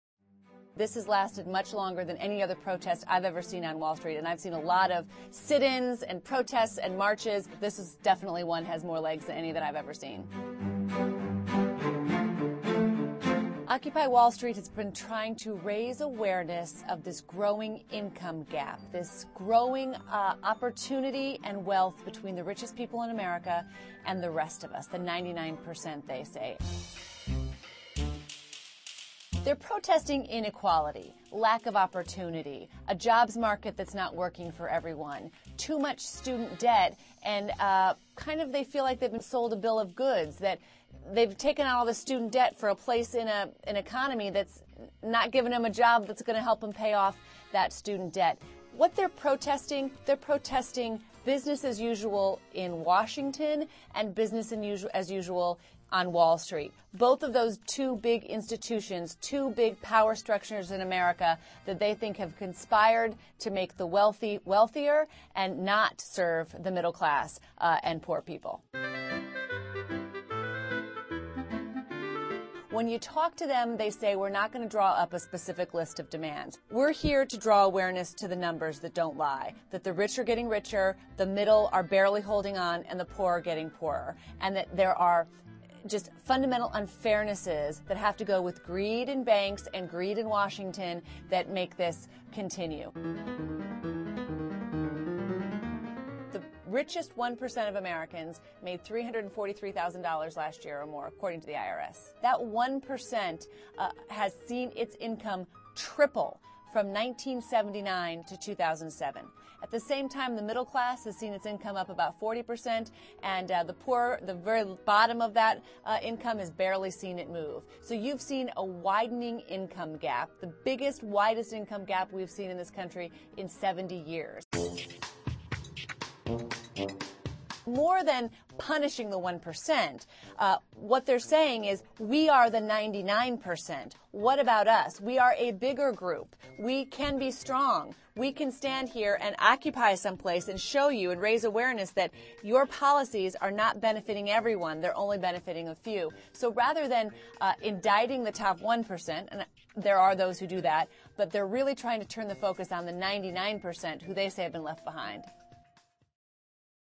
Explained: The OCCUPY Movement
The Occupy protests have entered their third month.

They have spread from New York around the world. You've probably heard about them, or run into them.
But you might not be sure who's protesting. Or why.

CNN's Christine Romans explains.